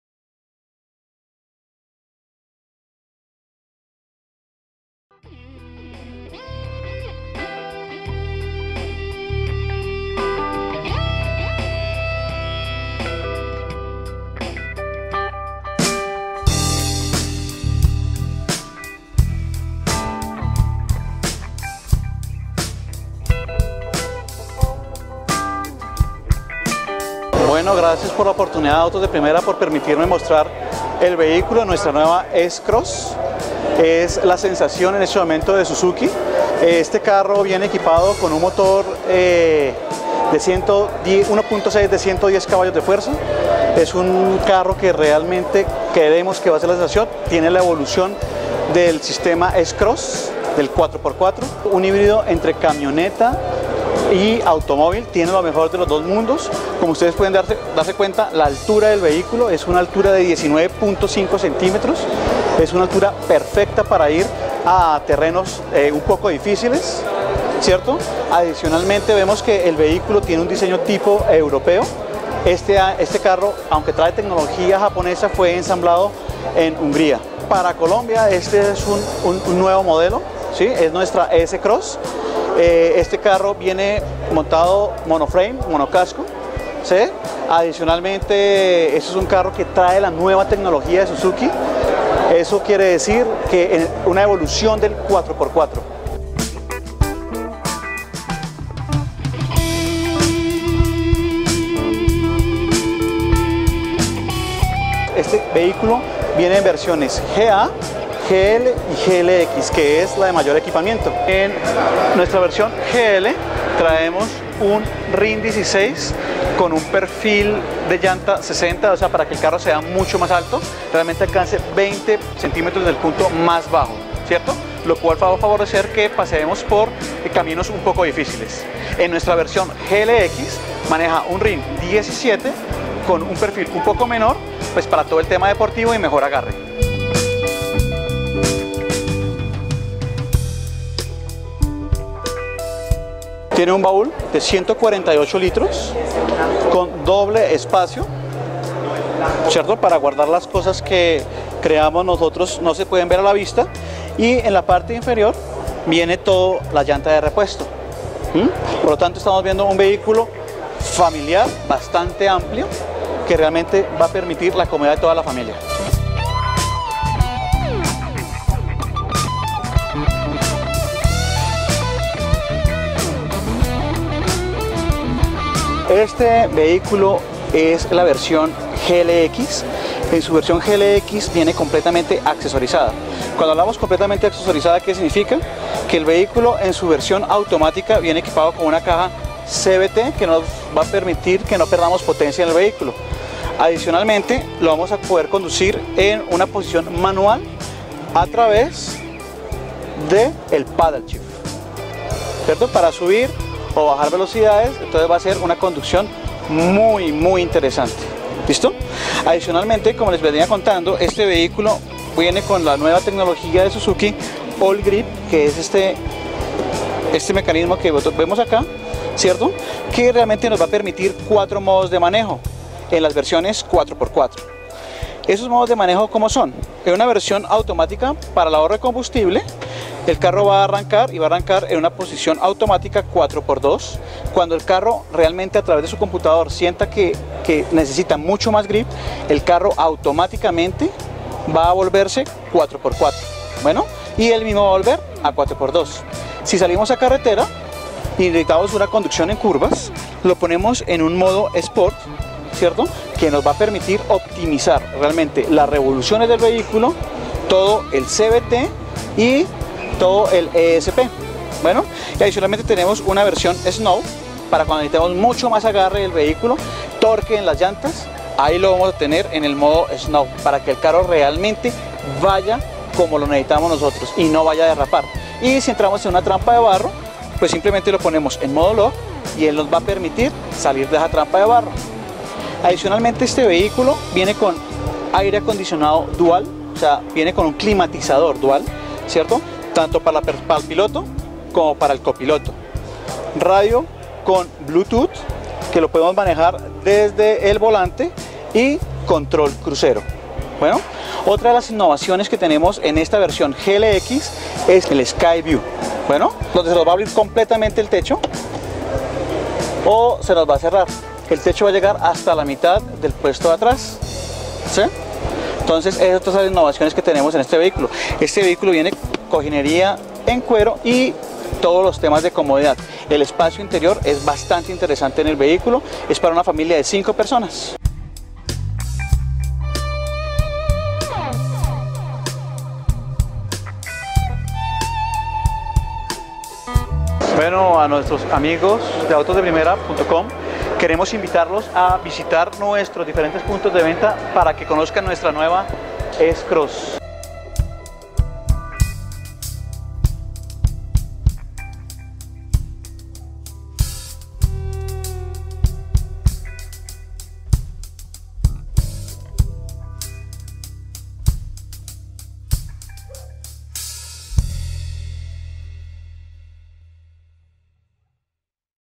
Nuevo Suzuki S-Cross en Colombia - Lanzamiento
Suzuki ya tiene disponible en Colombia el nuevo vehículo familiar crossover "S-Cross", llega con motor 1.6L de 118 caballos de potencia en versiones GA Mec 2WD, GL Mec y Aut 2WD, GL Mec y Aut All-Grip y GLX Mec y Aut All-Grip.